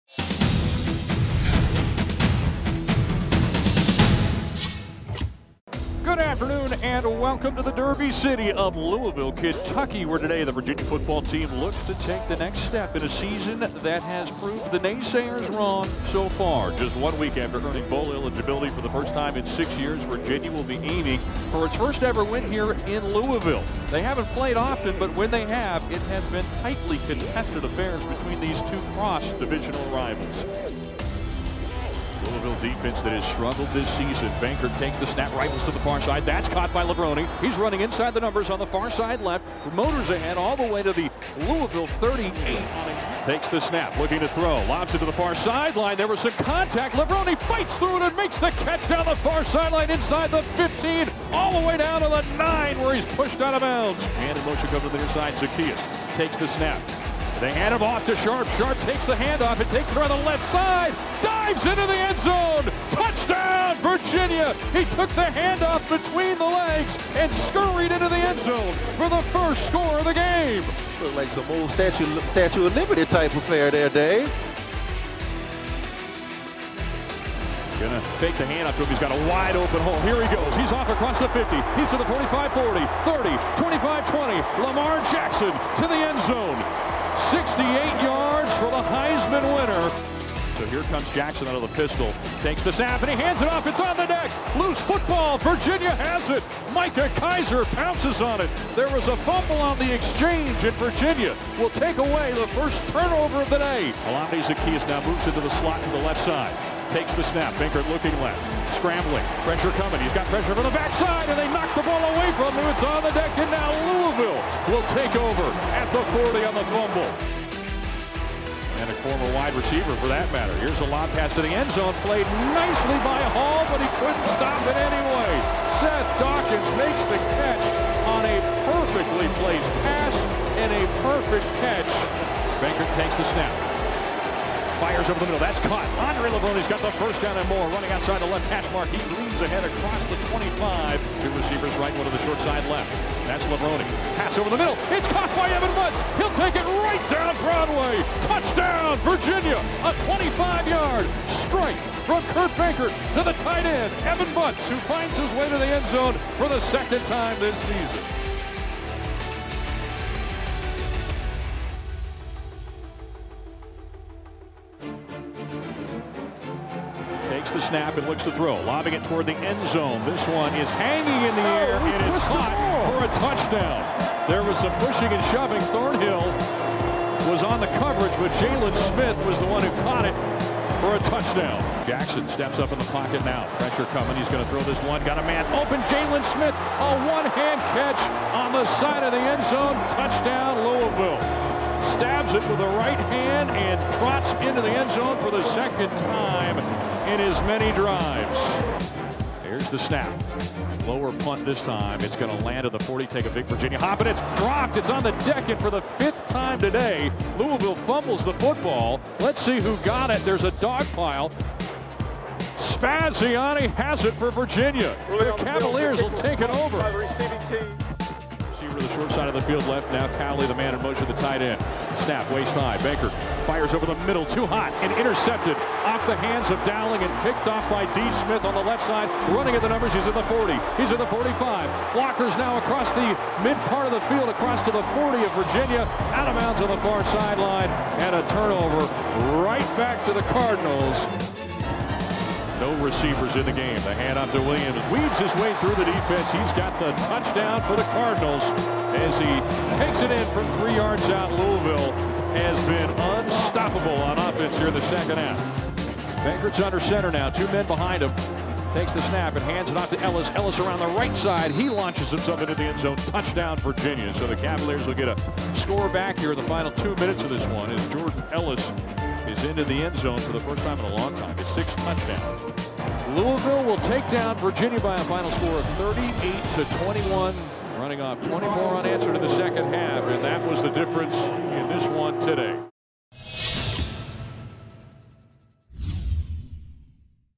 FOOTBALL - Louisville Highlights
Cards down the Cavaliers 38-21.